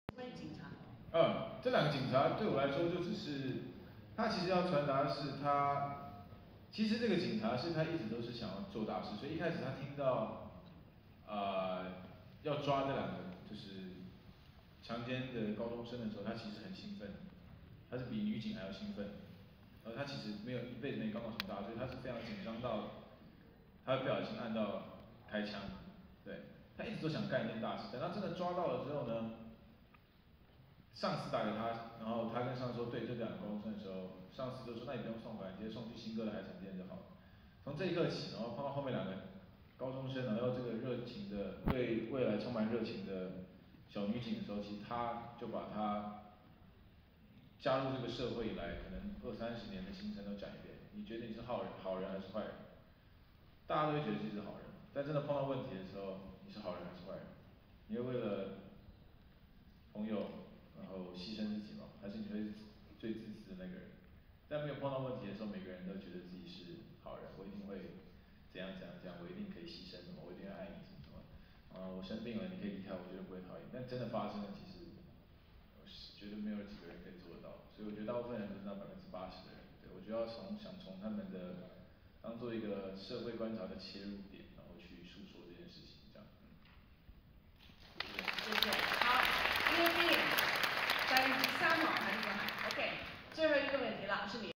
《黑的教育》HKIFF分享(柯震東/蔡凡熙)(part 6/7) @ 2023.04.04
《黑的教育》(Bad Education)
HKIFF分享(part 6/7)

日期：2023年4月4日（星期二）
場次：22 : 00（映後分享）
地點：香港 尖沙咀 香港文化中心 大劇院
嘉賓：導演柯震東、蔡凡熙

台灣發行： 禾豐九路娛樂
導演：柯震東
主演：蔡凡熙、朱軒洋、宋柏緯
台灣上映日期：2023年3月10日

2022 / 國語 / 77 分鐘 / DCP / 彩色 / 中文字幕

電影簡介：
一個月黑風高的晚上，三個高中畢業的男生，剛要脫離體制教育，就遇到「社會上好人還是壞人多？」的生存考題。天台上，他們交換說出駭人聽聞的秘密；街頭中，勇者遊戲玩太大，徹夜狂奔，卻逃不過隱藏Boss 的震撼教育。血與骨的成人禮後，黑夜將再來。柯震東演而優則導的初試啼聲，九把刀為兄弟度身訂造的劇本，視覺風格和演員調度都掌控自如，有過恣意輕狂的態度，更能領悟後青春的痛快。年輕演員朱軒洋獲金馬獎最佳男配角殊榮。